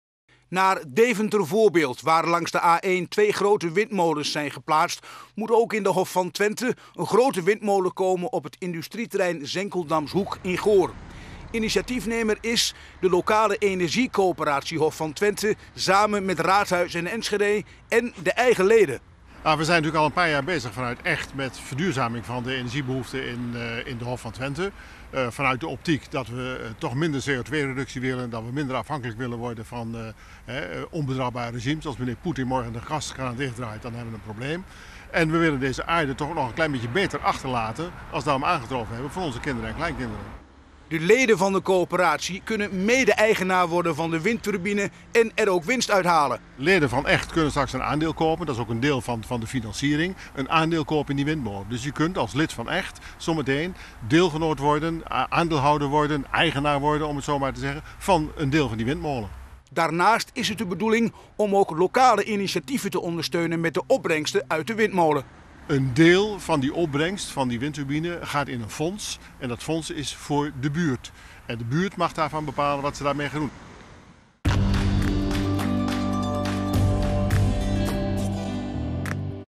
Plannen voor grote windmolen op Goors bedrijventerrein
Er zijn plannen voor de bouw van een grote windmolen op het Goorse bedrijventerrein Zenkeldamshoek.